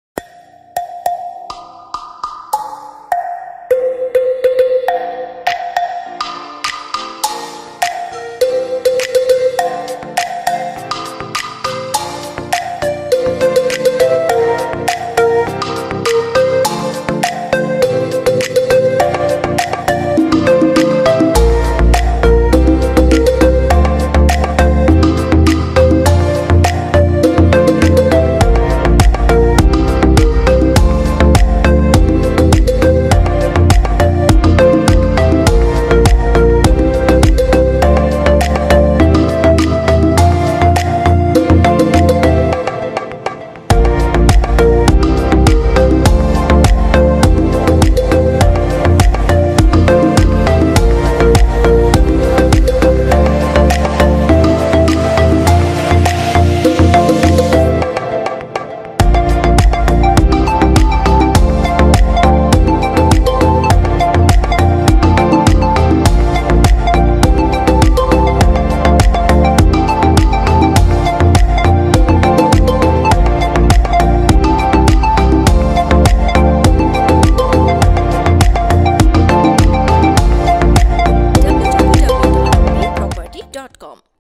Pleasant 500 Sq. Ft. Office in Jamal Khan | Commercial Space for Rent in Chattogram
This commercial office has a moderate capacity that you will find cozy. When you arrange this space according to the way you want, you will be able to carry out your business activities at ease. The rent is very affordable and you will be able to make a quick decision regarding the property. The space has been designed to ensure that you are able to meet your business needs very easily within the capacity that has been provided. for more info or call